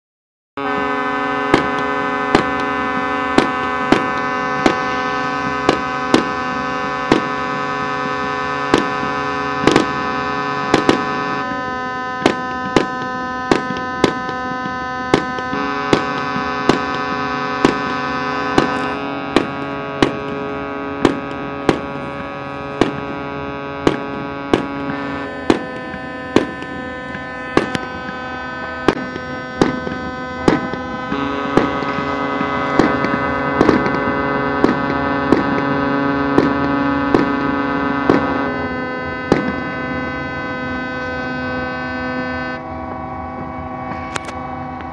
Fešta u Splitu
Proslava srebrene medalje Stipaničeva